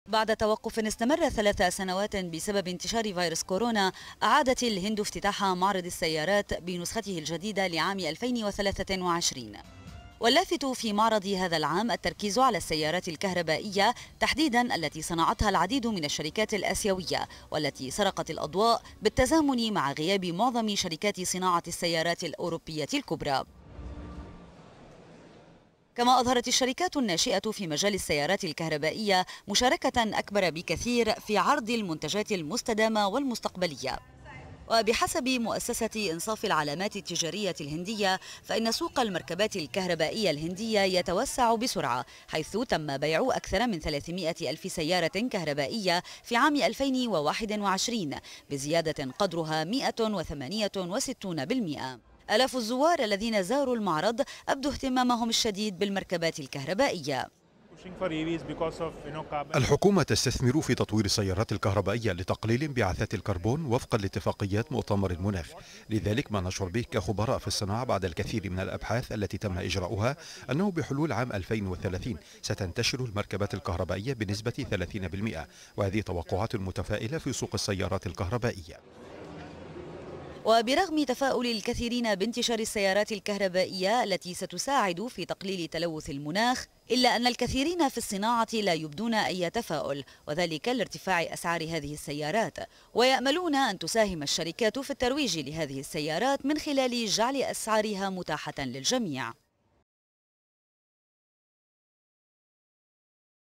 معرض السيارات في الهند يستقبل الزوار بعد 3 سنوات من توقفه.. ما اللافت هذا العام
أصبح تغير_المناخ قضيةً رئيسية بالنسبة لـ الهند ، مع تزايد موجات الحرارة والمخاوفِ بشأن تلوث الهواء الذي يؤثر على ملايين الأشخاص.
تتولى الهند حاليًا رئاسةَ مجموعةِ العشرين للاقتصادات الرائدة ، حيث تضع المناخ على رأس أولويات المجموعة.